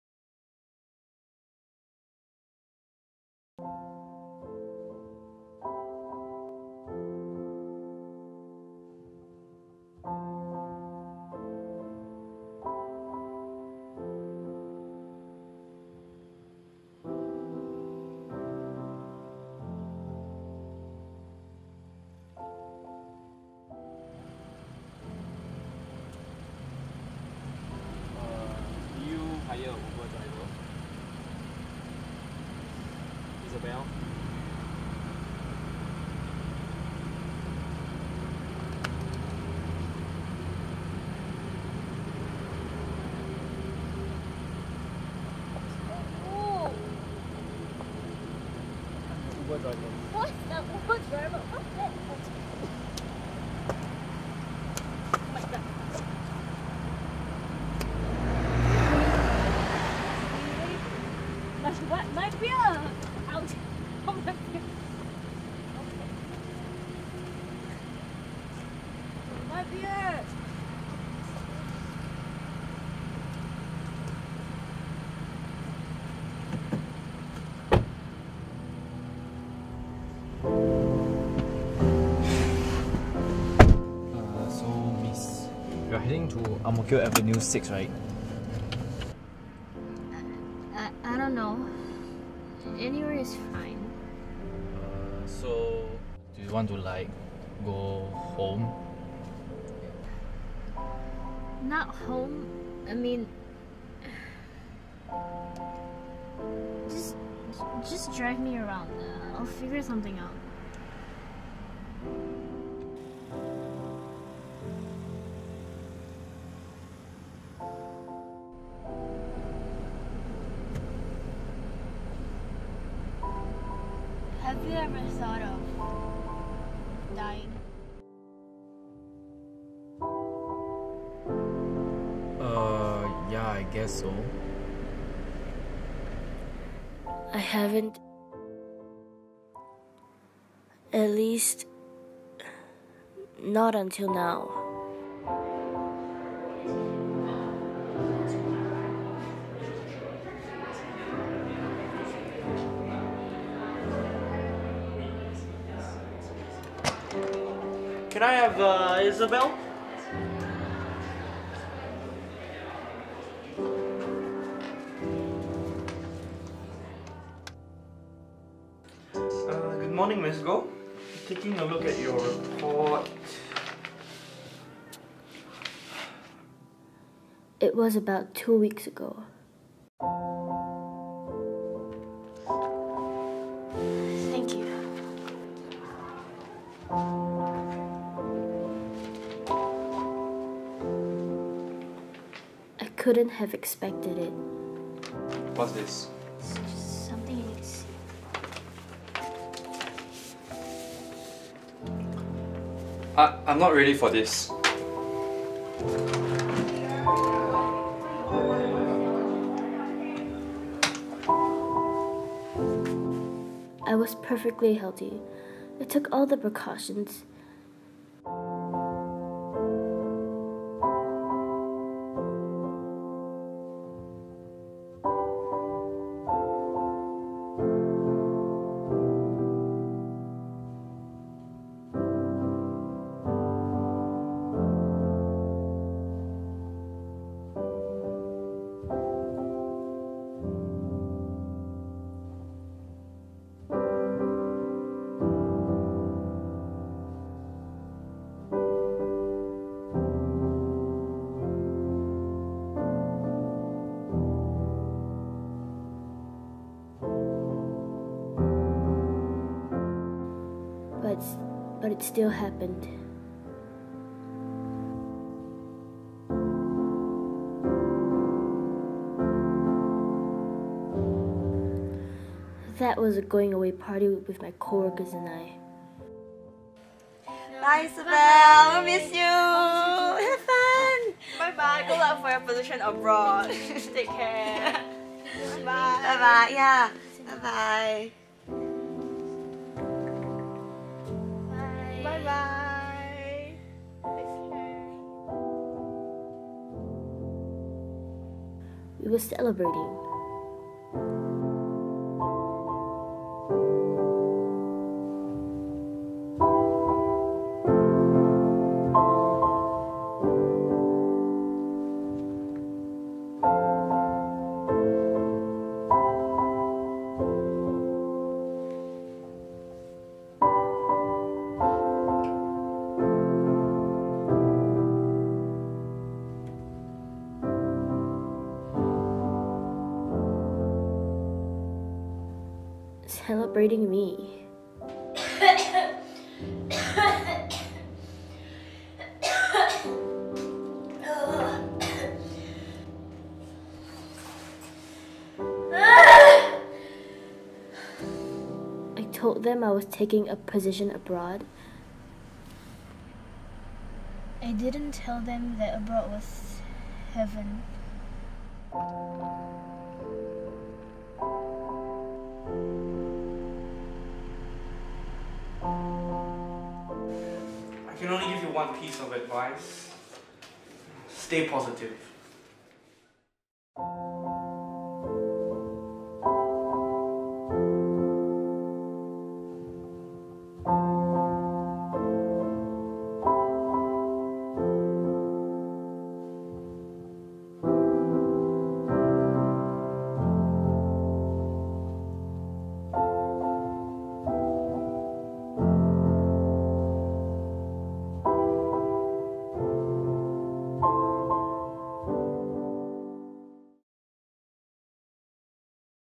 The Last Ride
This is a short film by my group of friends and I from Ngee Ann Polytechnic Mass Communication.